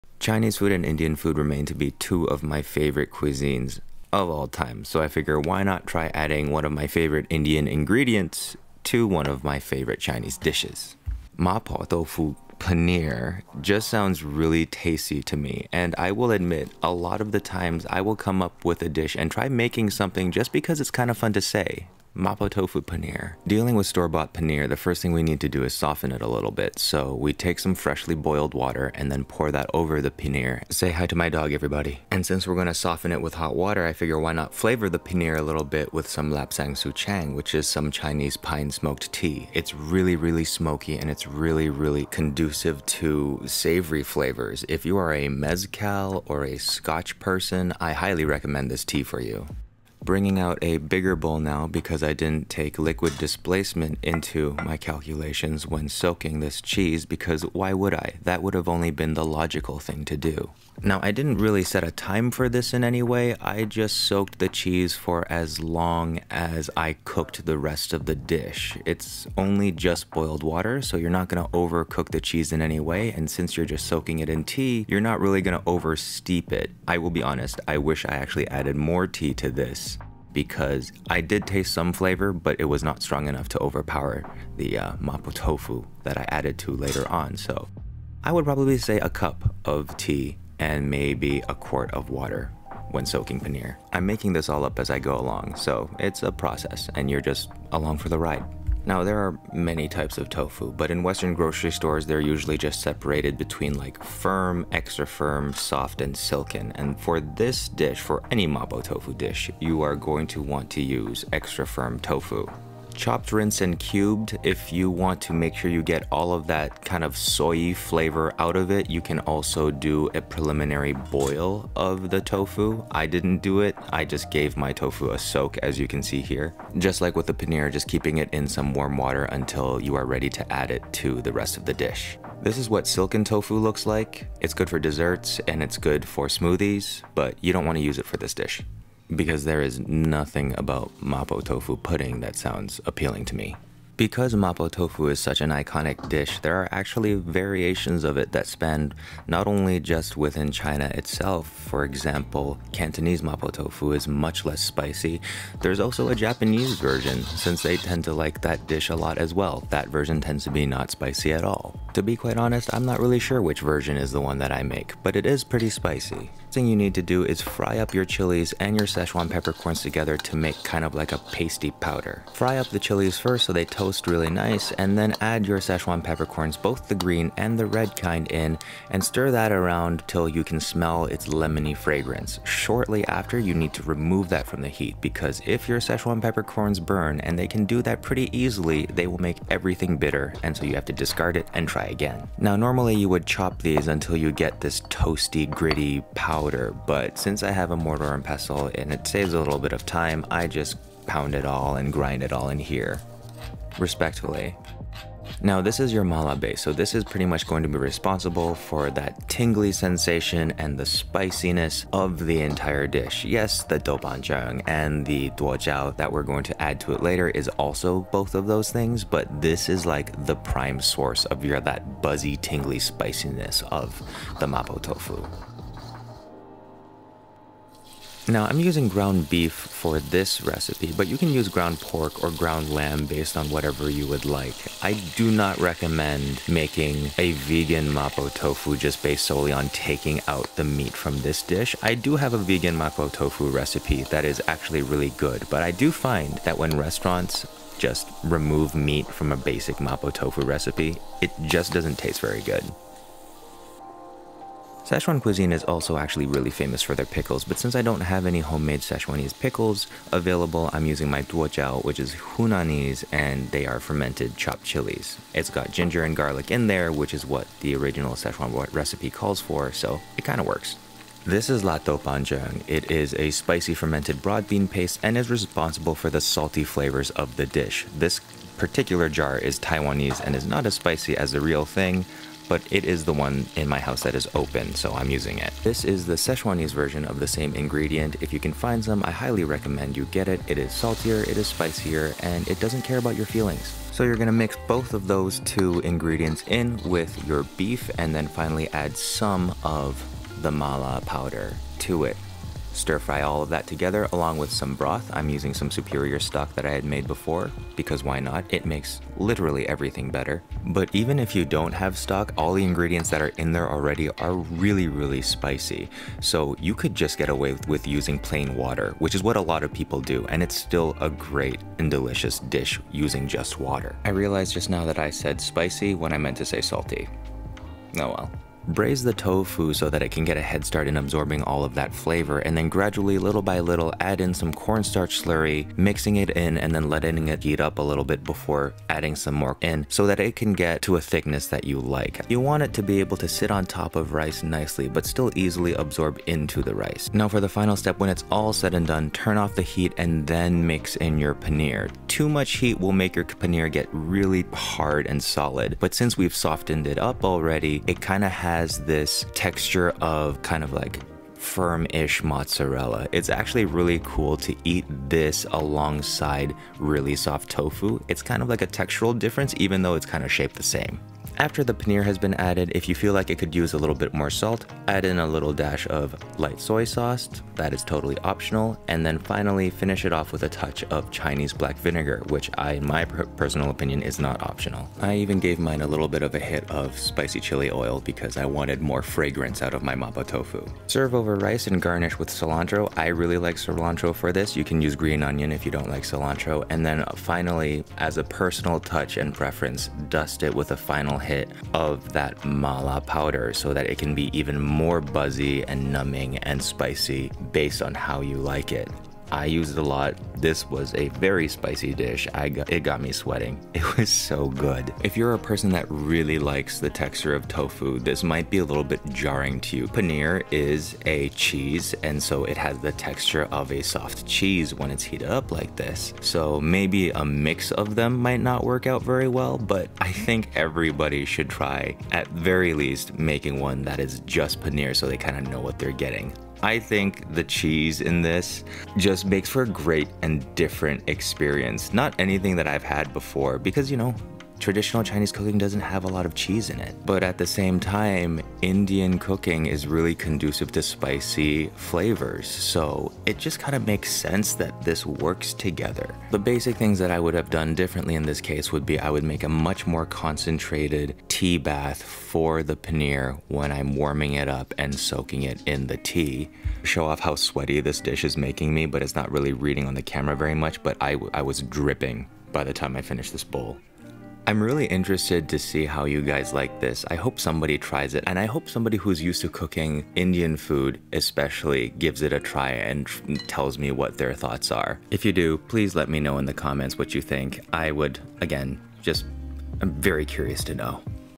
Chinese + Indian: Mapo Tofu with Paneer? (and ground beef)
Chinese and Indian food are two of my favorite cuisines, which is why I decided to combine them to make mapo tofu paneer with ground beef. The version that I made was very spicy but it doesn't have to be if you're not a fan of spicy food. For those of you who wish to try making this at home, here are the steps:

1 block extra firm tofu
1 block paneer
1/2 lb ground beef
1 cup dried chilies
1/8 cup red Szechuan peppercorns
1/8 cup green Szechuan peppercorns
1 tbsp doubanjiang
1 tbsp duo jiao
3 cups water or broth
Corn starch slurry
Lapsang souchang tea

Directions

Soak the cubes paneer in hot water with lapsang souchang tea

Rinse and cube the tofu

Toast and chop the chilis and peppercorns

Cook the ground beef

Add the two chili pastes and mix before adding broth and tofu.

Simmer and the thicken with slurry

Turn the heat off and add paneer

Garnish and serve over rice